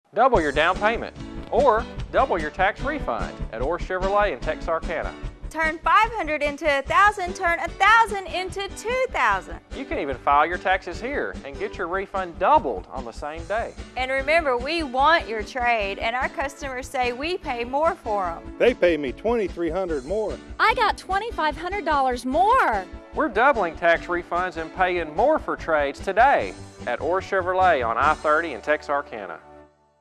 Orr Chevrolet Double Your Refund
30 second TV commercial for Orr Chevrolet in Texarkana, TX. Produced by The Guerra Group. Joel Orr and Jan Orr on-camera talent. Patrick Guerra  writer. Tim Taylor editor.